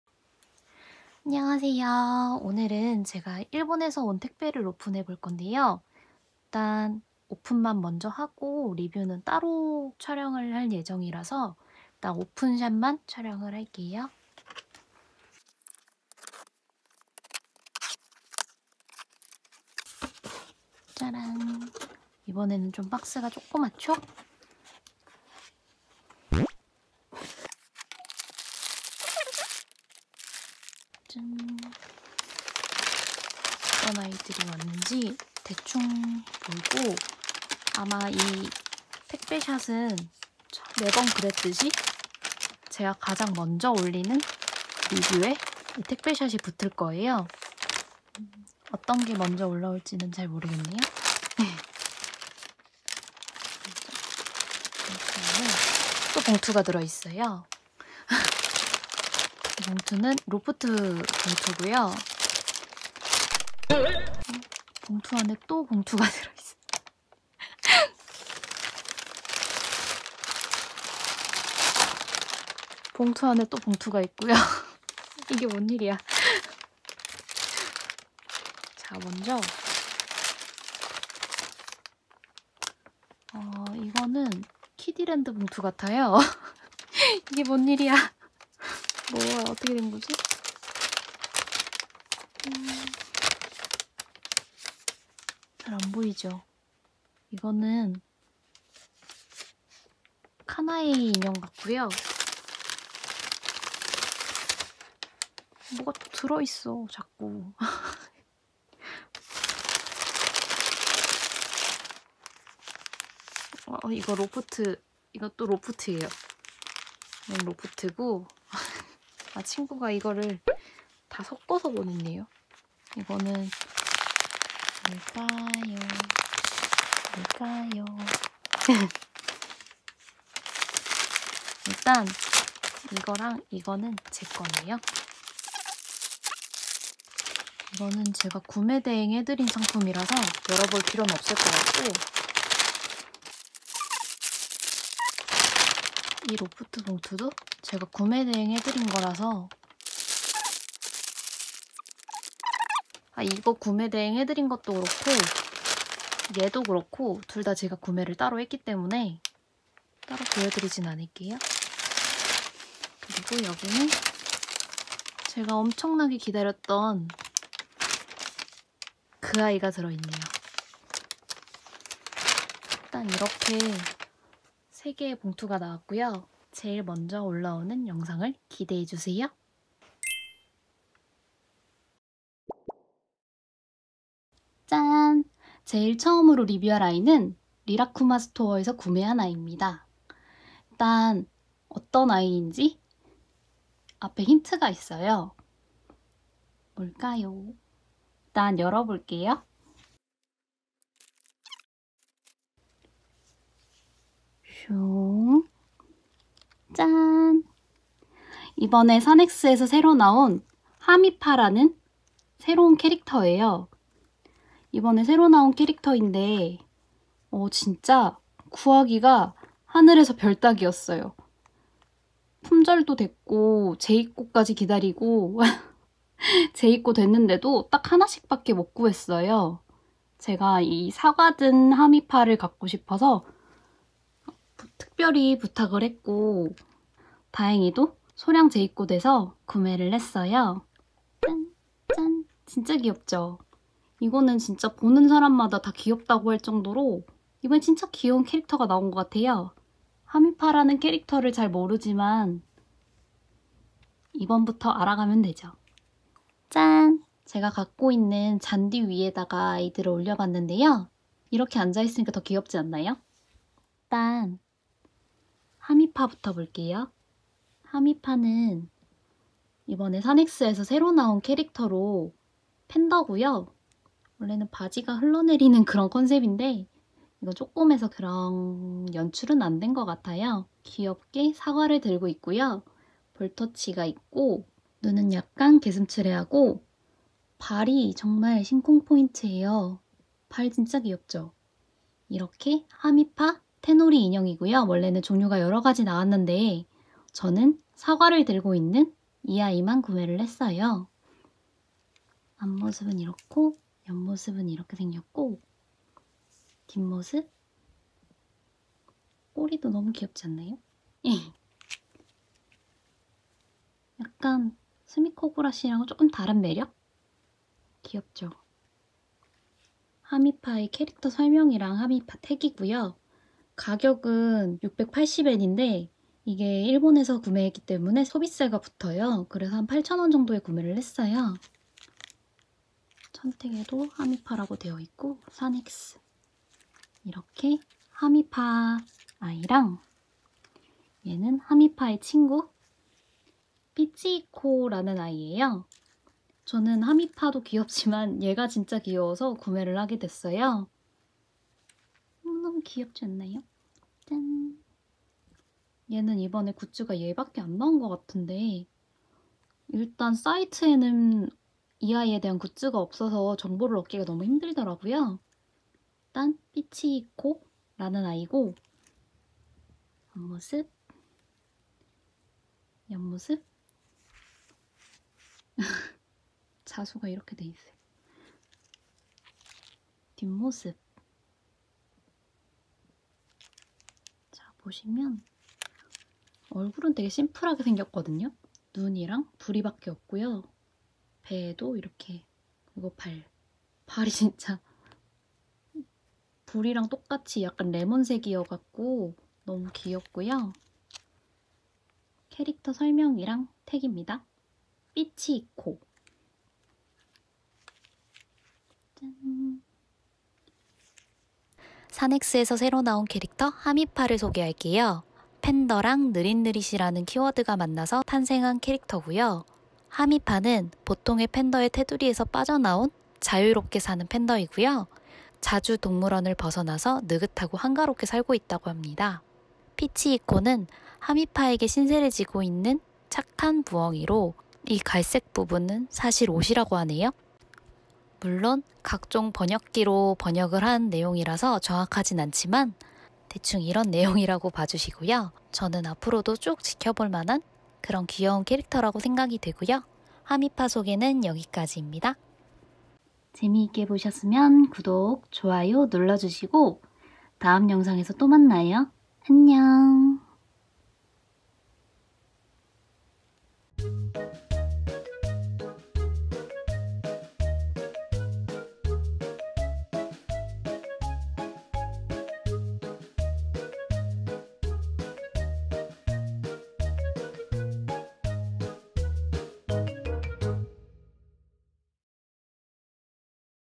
스밋코구라시 친구🤔? 산엑스 새캐릭터 하미파를 소개합니다!
안녕하세요 키덜트빈입니다
산엑스에서 나온 새로운캐릭터!
'하미파'를 소개합니다!
3월에 새로나온 캐릭터라서 아직 정보가 없어서🤔
직접 번역한💖 캐릭터 설명까지 넣었어요
귀여운 팬더 🐼하미파에게 빠질 준비하세요!
메인 캐릭터보다 더 귀여운 부엉이,🦉
피치히코도 구매 했답니다 :)
다음에 올라올 리뷰도 사알짝! 스포가 있어요!

재미있게 보셨다면,
구독! 좋아요! 부탁드리고
인스타, 블로그에서도 만나요 🌿